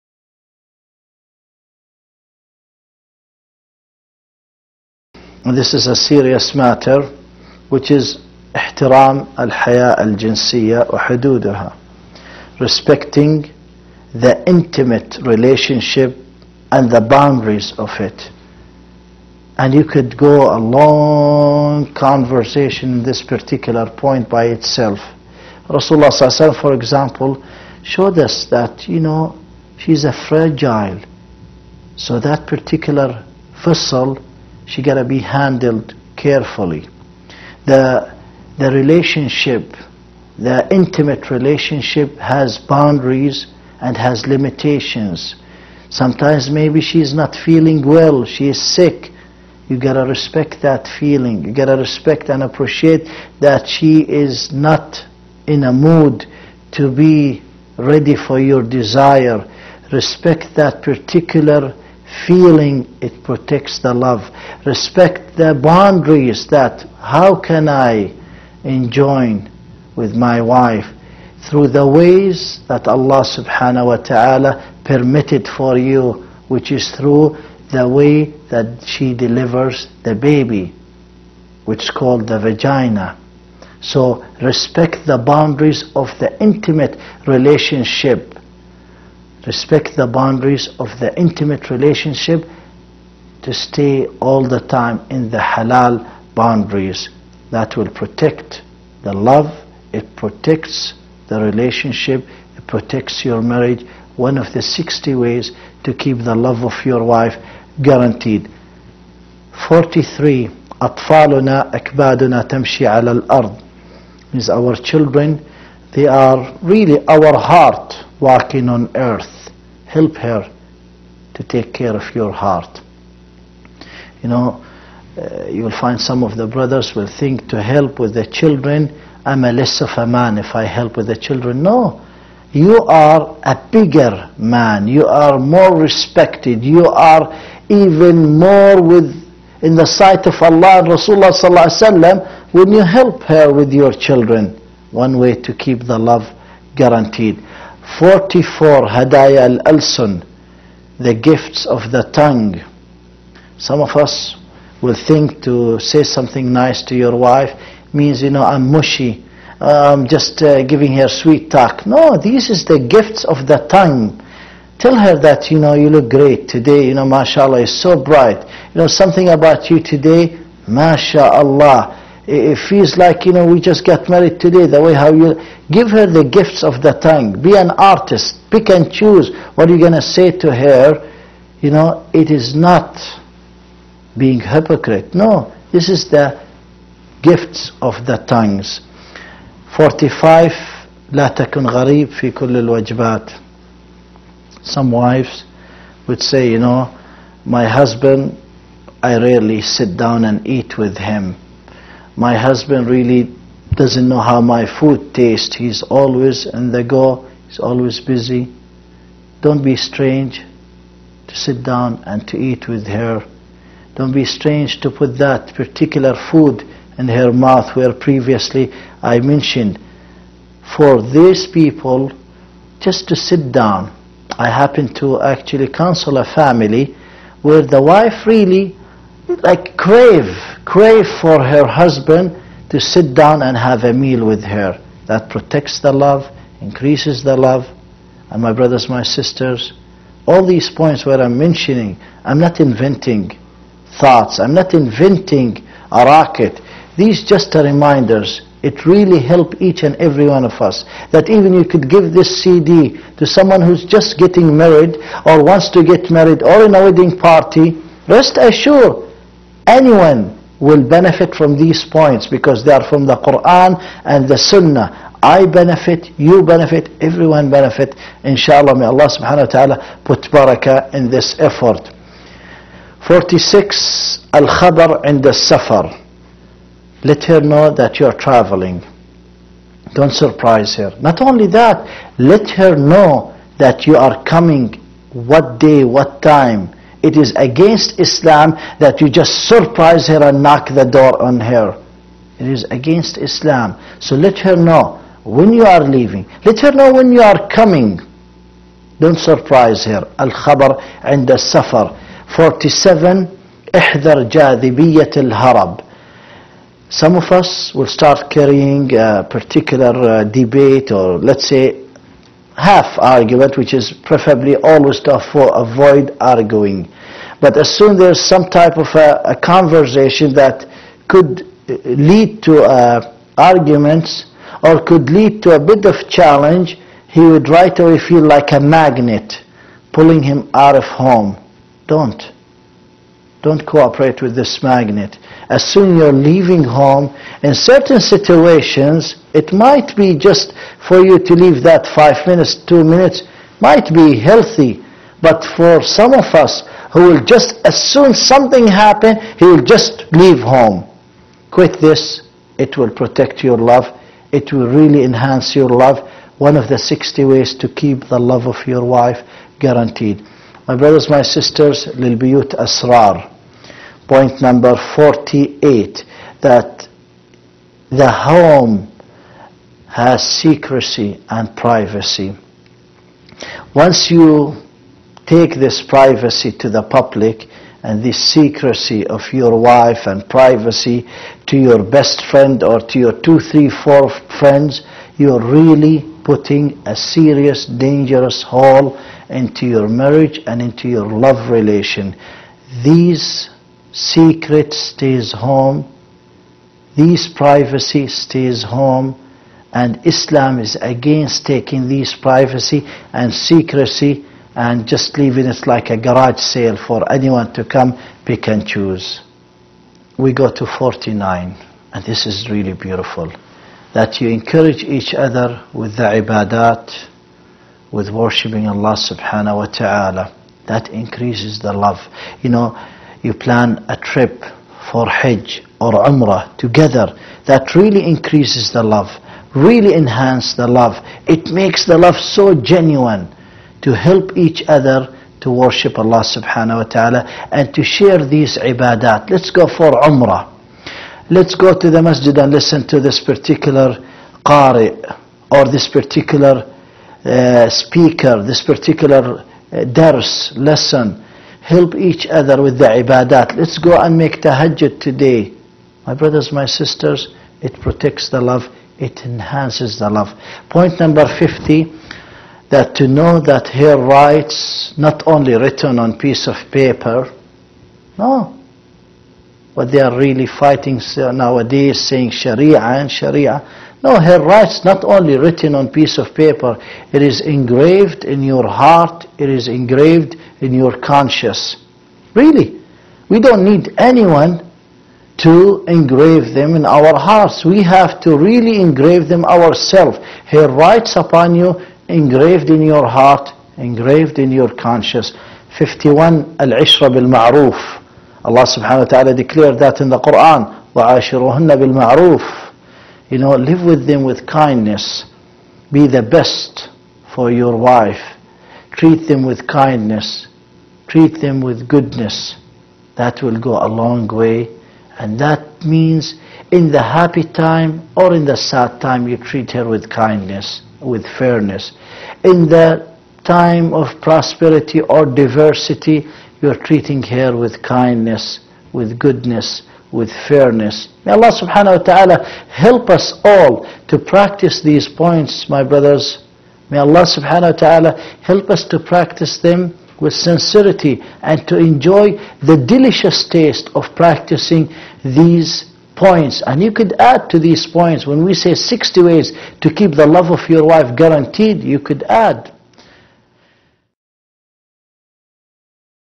How to Love Your Wife (Part 5)
60 Ways to Keep Your Wife's Love - Guaranteed!

How to Keep Your Wife's Love (Part 5)
Ways number 42 to 51 are shared here.